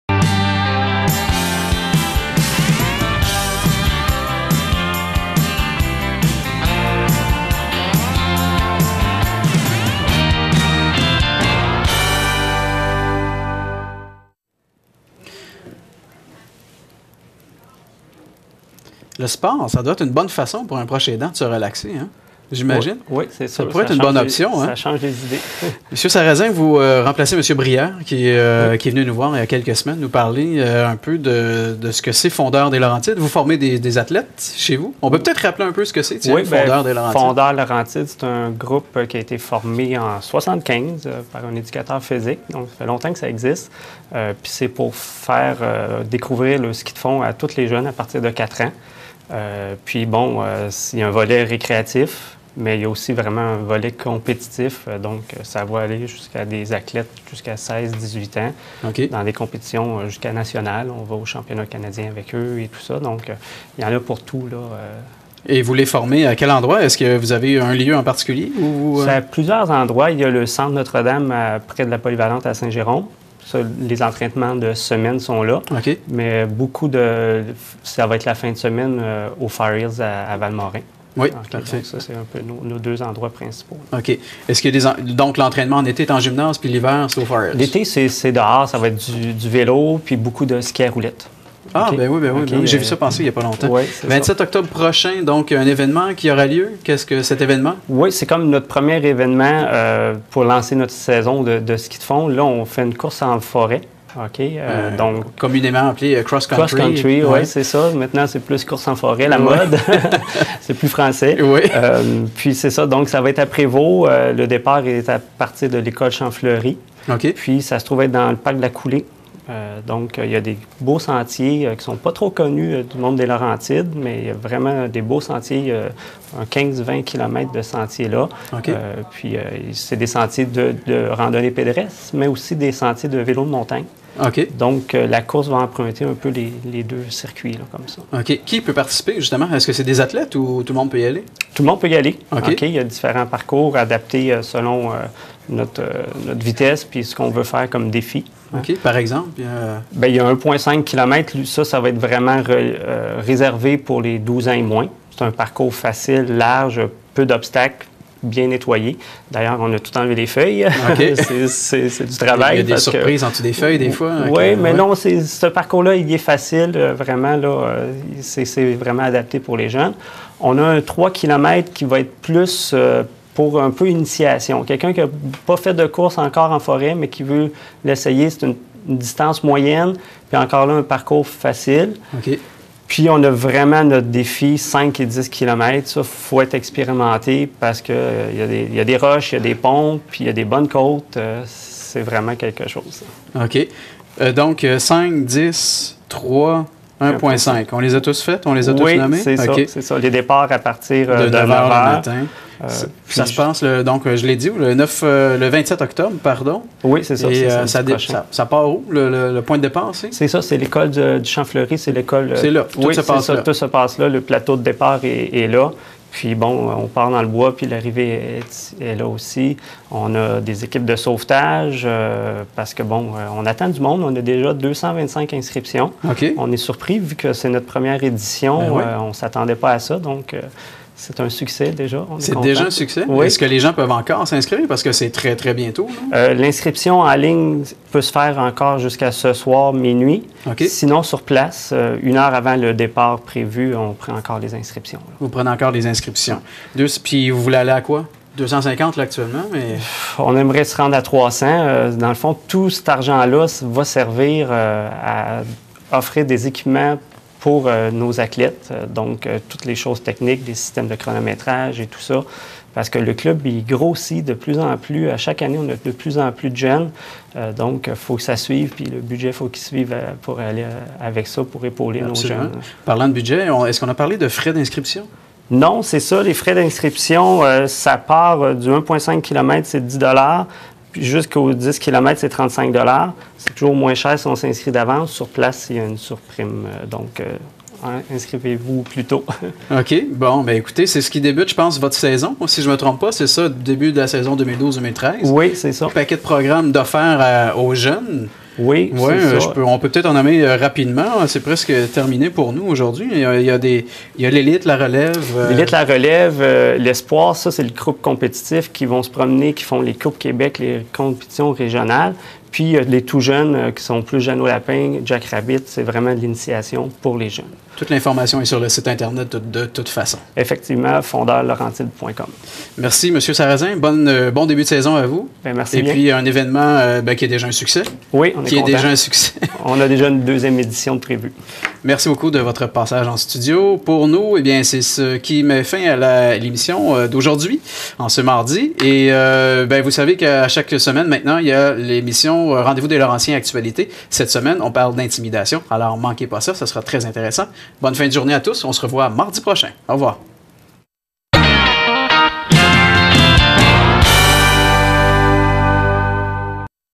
Fondeurs-Laurentides
DOMINIC REÇOIT: Pierre Sarrazin, Menbre du comité organsateur, Fondeurs-Laurentides